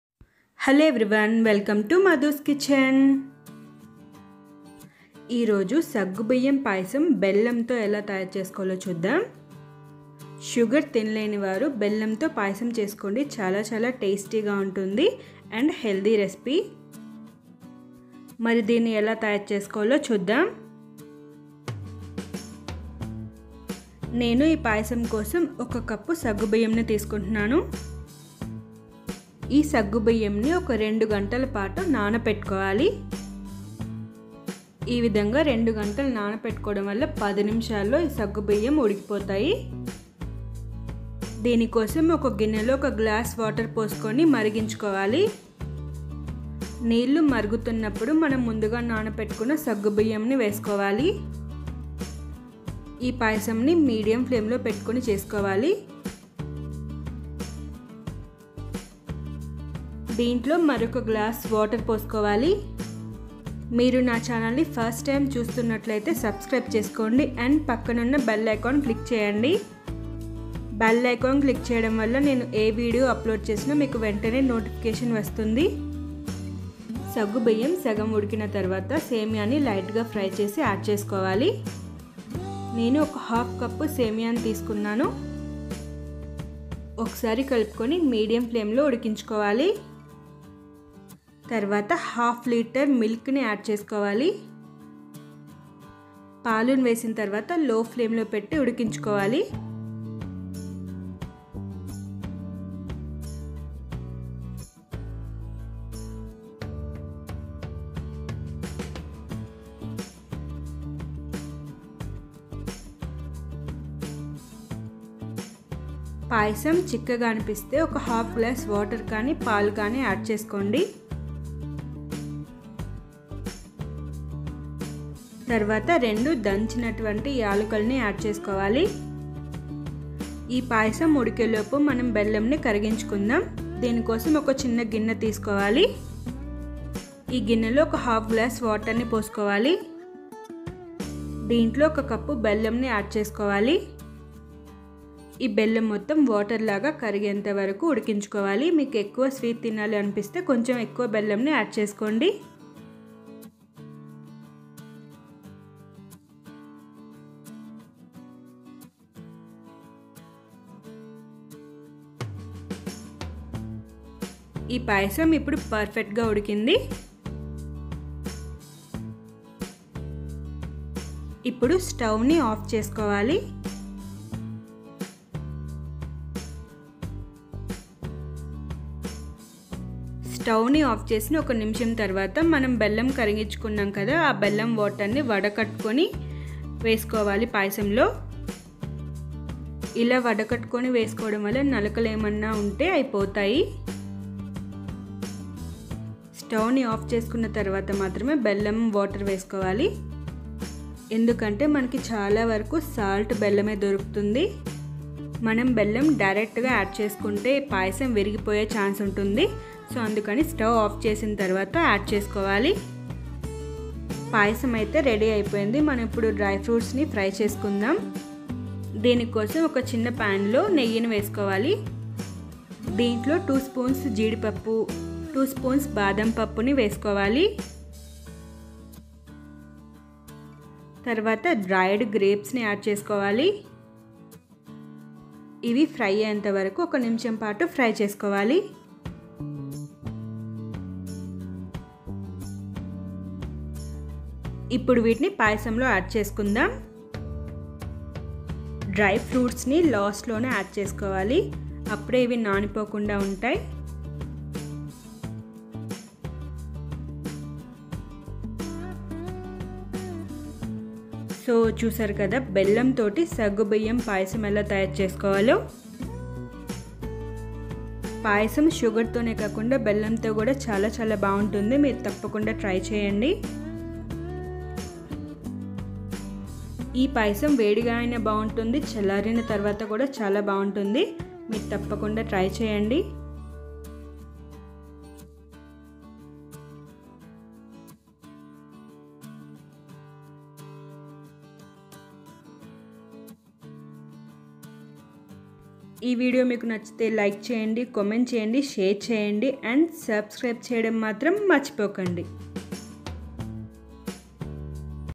ఎండాకాలంలో శరీరాన్ని చల్లపరిచే బెల్లం తో సగ్గుబియ్యం పాయసం || Saggubiyyam Payasam || Sabudna kheer
బెల్లం తో సగ్గుబియ్యం పాయసం || Saggubiyyam Payasam  || Sabudna kheer With jaggery in telugu